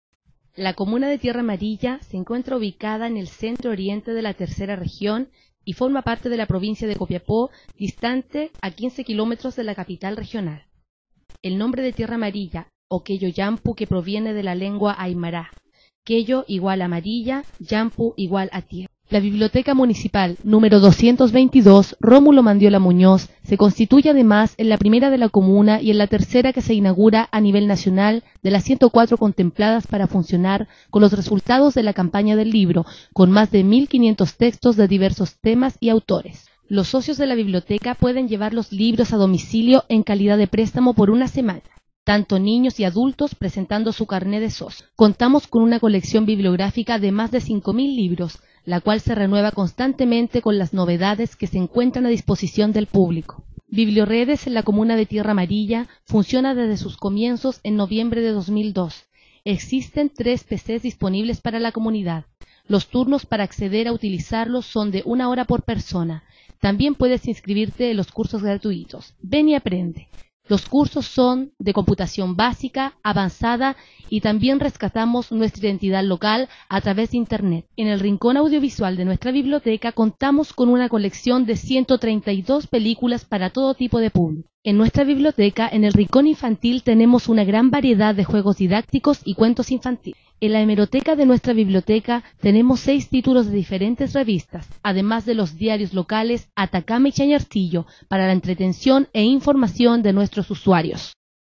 Presentación Biblioteca Tierra Amarilla
Video en que se presentan los diferentes servicios que se ofrecen en nuestra Biblioteca Pública Romulo Mandiola Muñoz de la comuna de Tierra Amarilla, en la Tercera Región de Atacama.